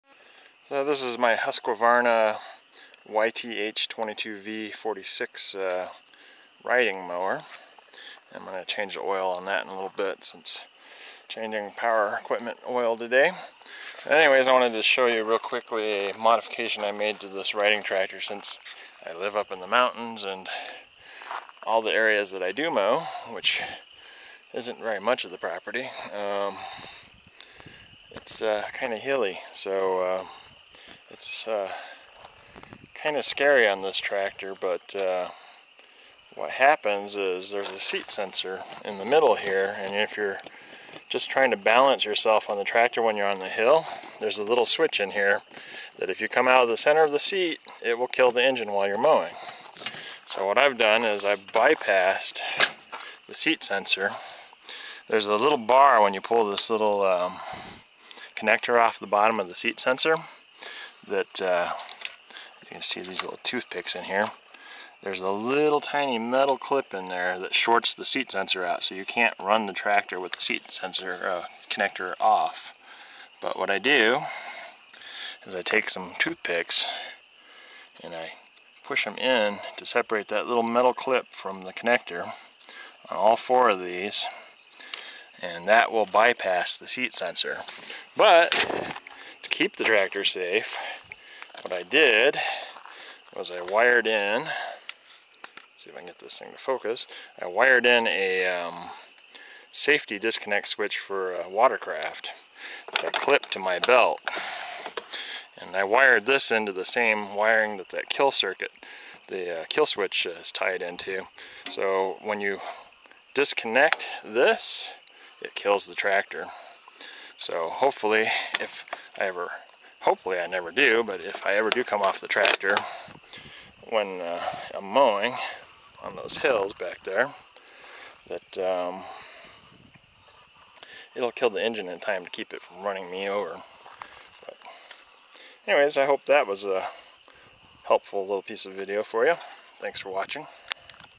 Husqvarna Riding Mower Seat Safety Kill Switch Modification
Here is the kill switch on Amazon
When I am mowing up here on my property in the mountains there are plenty of spots that I have to shift in my seat to feel balanced when I am going up and down hills. The Husqvarna seat  safety switch will kill the engine if you come out of the center of the seat while mowing, so I modified my riding tractor with a watercraft kill switch to stop the engine if I come off of the tractor.